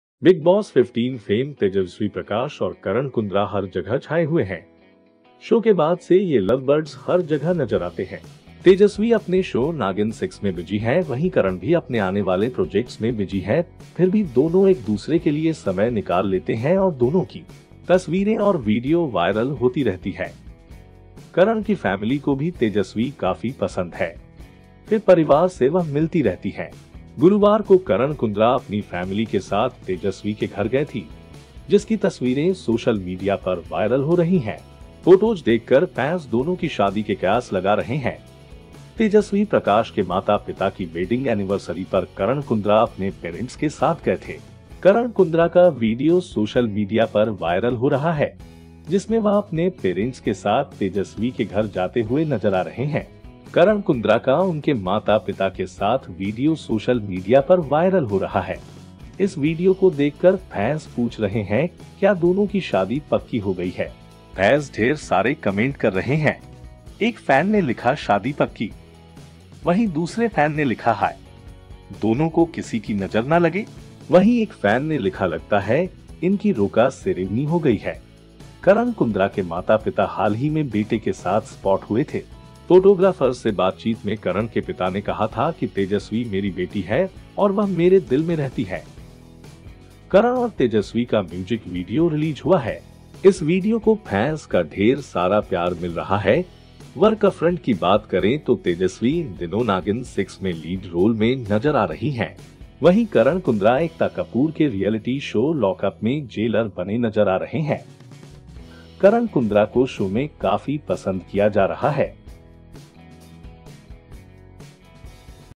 करण कुंद्रा अपने Paresnt के साथ गए तेजस्वी प्रकाश के घर, Fans ने पूछा- शादी पक्की
करण कुंद्रा अपने पेरेंट्स के साथ गए तेजस्वी प्रकाश के घर, फैंस ने पूछा- शादी पक्की
करण कुंद्रा के घर अपने माता-पिता के साथ आधी रात में पहुंची तेजस्वी प्रकाश, क्या हो रही है शादी की प्लानिंग?